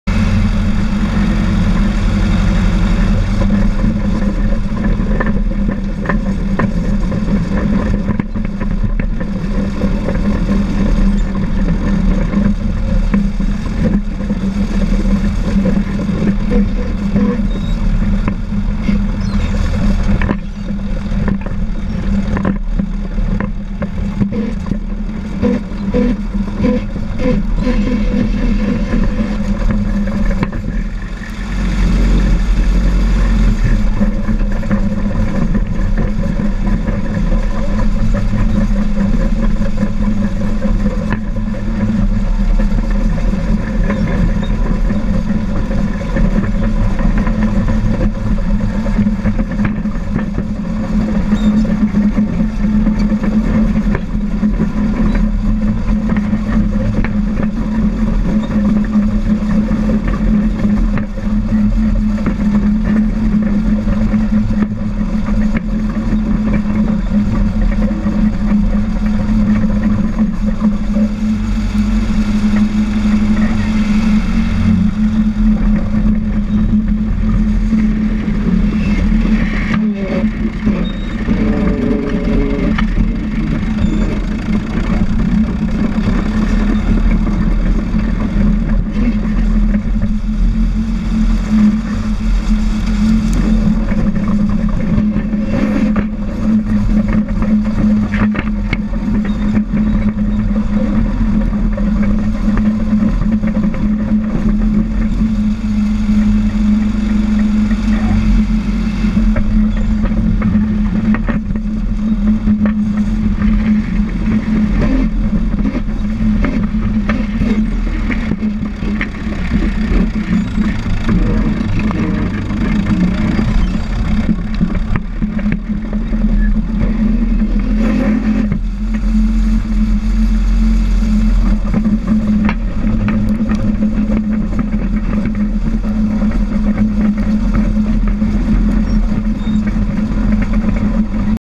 Badger Steam Gas D4 Cat #1
2014 Badger Steam and Gas Show - Baraboo,WI - Kurt pushing dirt in the D4 7U Caterpillar